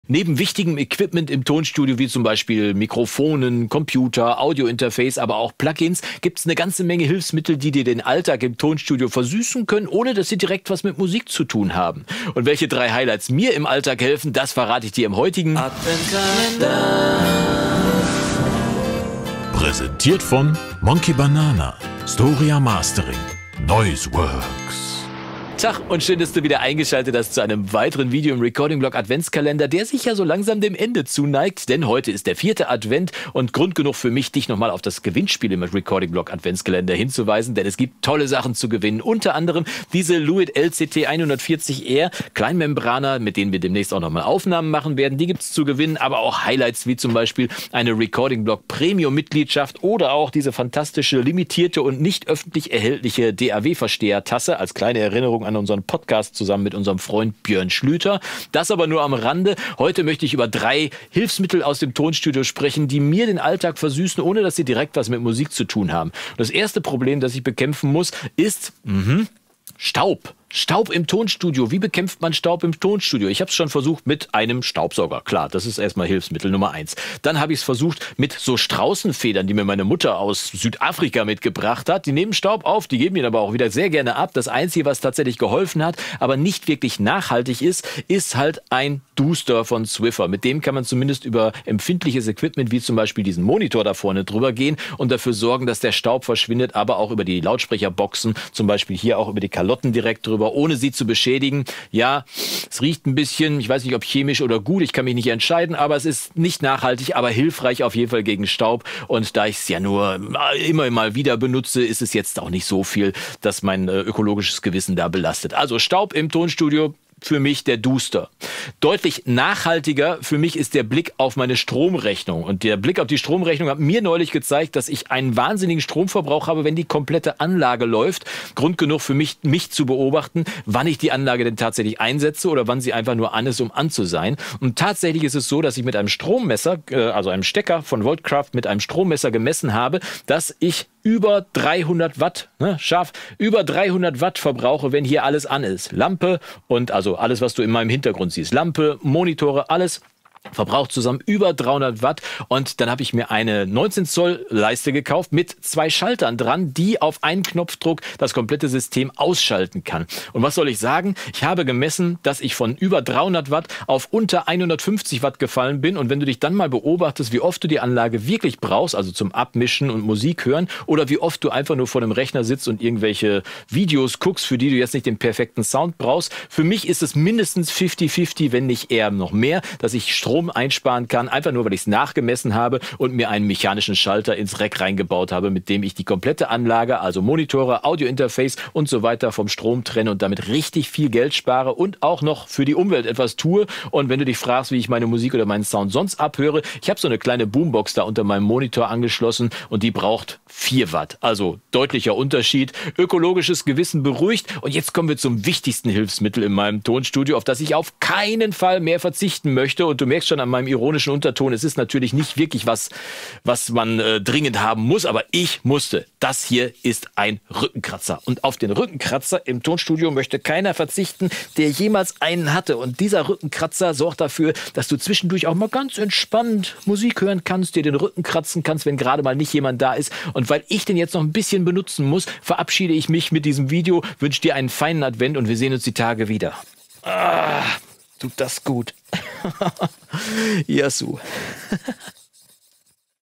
Für mich unverzichtbar im Tonstudio | Recording-Blog Adventskalender 18
Adventskalender 18: Unverzichtbare Hilfsmittel im Tonstudio müssen nicht unbedingt etwas mit Musik zu tun haben, können aber trotzdem wichtig sein. Auf diese 3 verzichte ich nicht mehr!